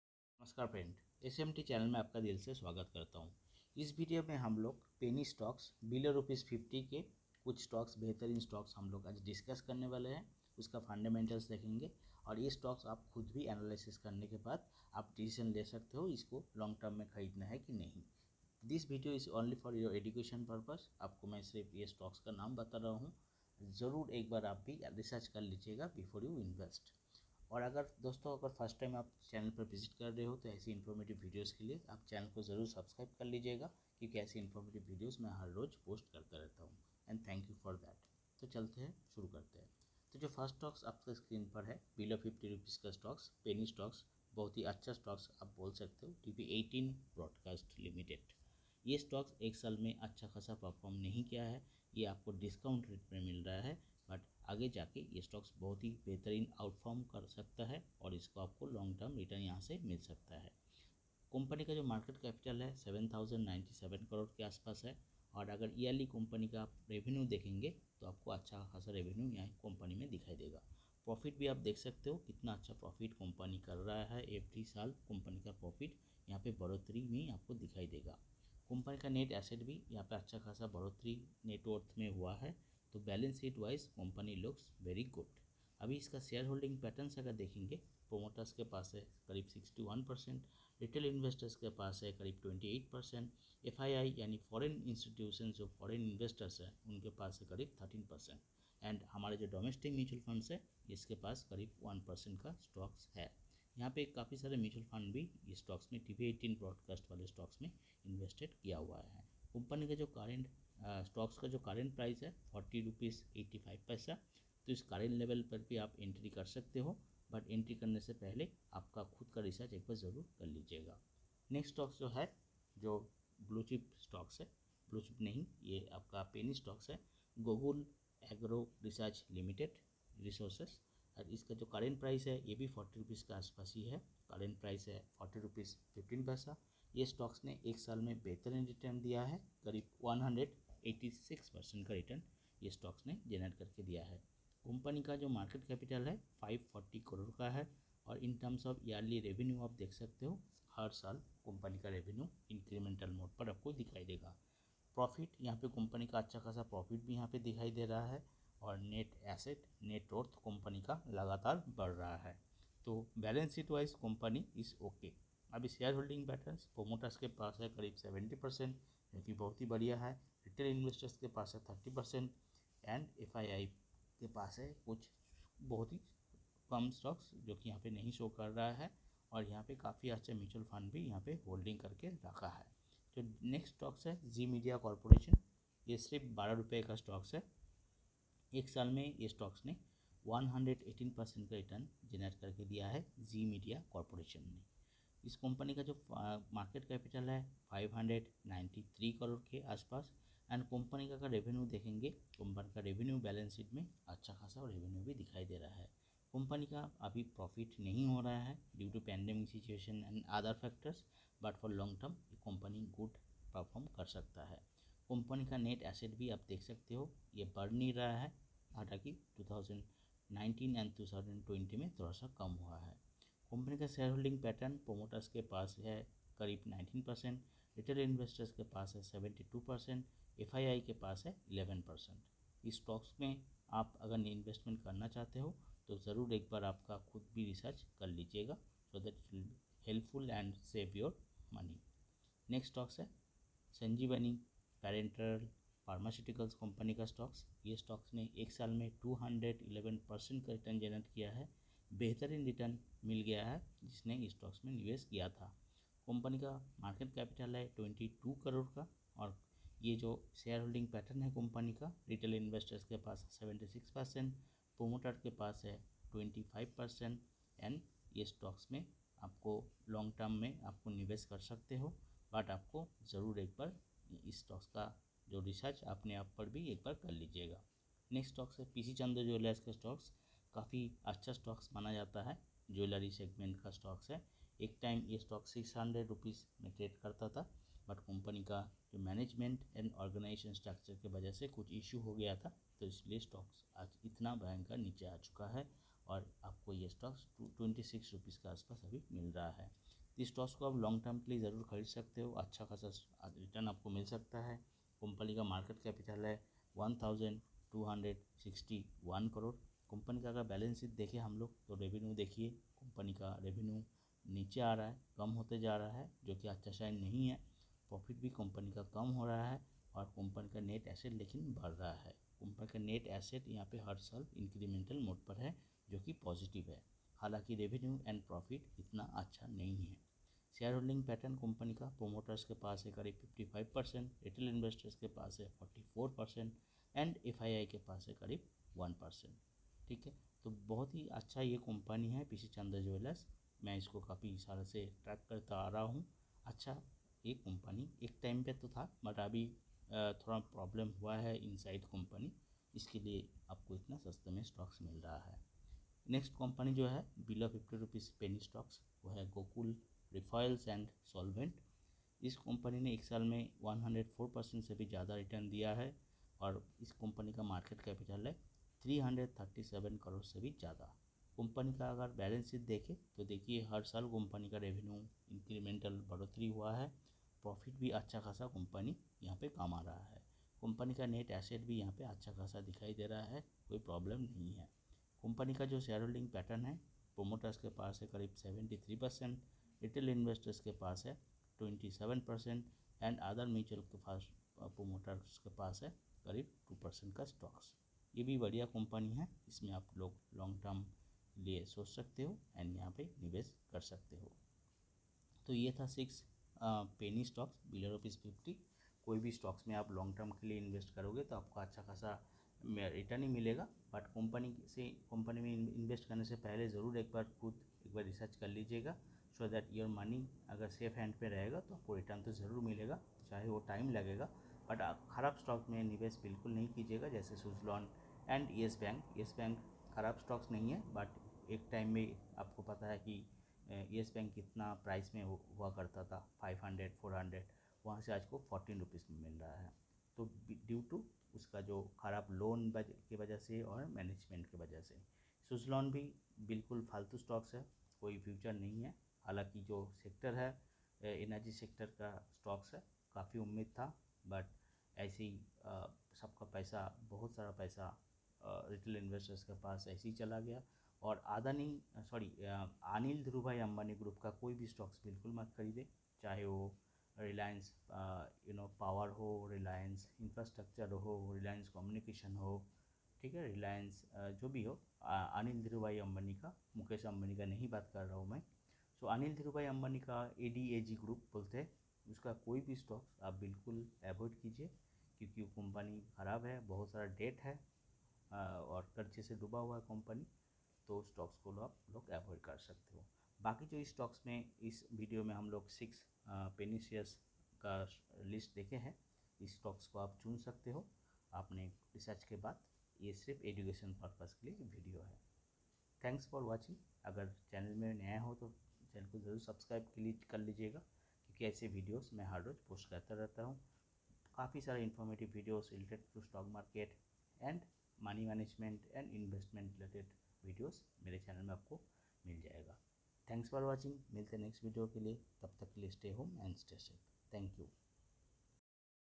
List of 6 Penny Stocks Below Rs 50
Gokul Refoils & Solvent,PC Jeweller,Sanjivali Paranteral,Zee Media Corp,Gokul Agro Resources, TV18 Broadcast.

Smart Market Tips.